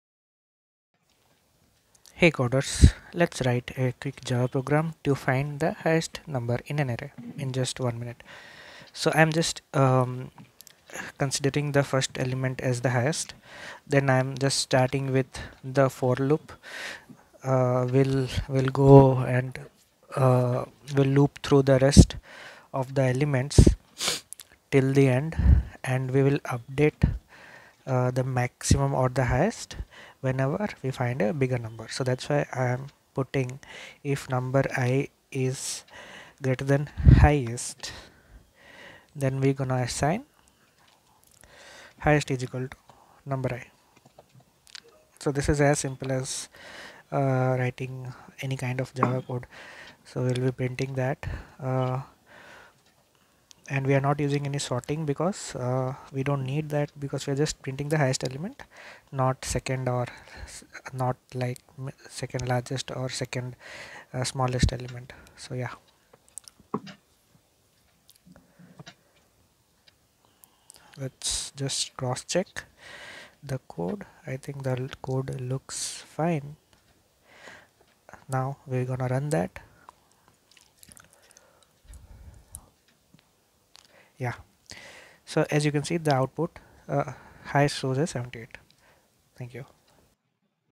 1. Print the highest number from an array | Java Program series | QA Automation with Gaurav
Logic behind finding the largest value
Clean Java code
No sorting required
Interview-friendly approach